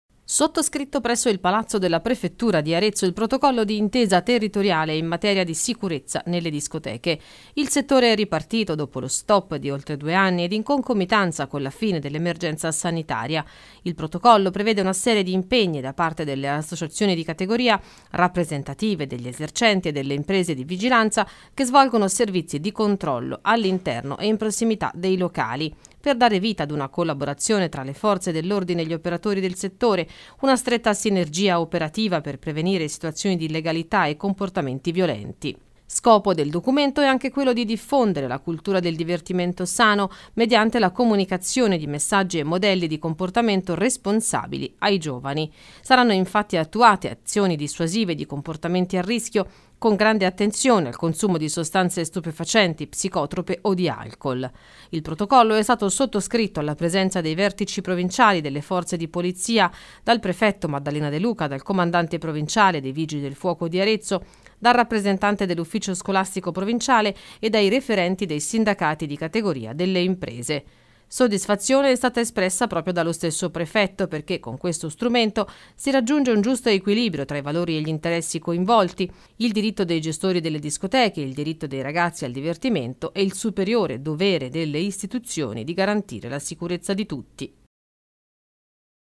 Sottoscritto il "Protocollo d'intesa territoriale in materia di sicurezza delle discoteche"
Sottoscritto presso il palazzo della Prefettura aretina il "Protocollo d'intesa territoriale in materia di sicurezza delle discoteche".
Il settore è ripartito da poco dopo lo stop di oltre due anni ed in concomitanza con la fine  dell'emergenza sanitaria.
Il protocollo prevede una serie di impegni da parte delle associazioni di categoria rappresentative degli esercenti e delle imprese di vigilanza che svolgono servizi di controllo all'interno e in prossimità dei locali, per dare vita ad una collaborazione tra le forze dell’ordine e gli operatori del settore. Una stretta sinergia operativa per prevenire situazioni di illegalità e comportamenti violenti.
Scopo del documento è anche quello di diffondere la cultura del "divertimento sano", mediante la comunicazione di messaggi e modelli di comportamento responsabile ai giovani.
Saranno infatti attuate azioni dissuasive di comportamenti a rischio, con attenzione al consumo di sostanze stupefacenti o psicotrope e di alcol.
Il Protocollo è stato sottoscritto, alla presenza dei vertici provinciali delle Forze di polizia, dal Prefetto Maddalena De Luca, dal Comandante Provinciale dei Vigili del Fuoco, dal rappresentante dell’Ufficio Scolastico Provinciale e dai referenti dei sindacati di categoria delle imprese.
Soddisfazione è stata espressa dal Prefetto di Arezzo Maddalena De Luca, perché con questo strumento si raggiunge un giusto equilibrio tra i valori e gli interessi coinvolti: il diritto dei gestori delle discoteche, il diritto dei ragazzi al divertimento, il superiore dovere delle istituzioni di garantire la sicurezza di tutti.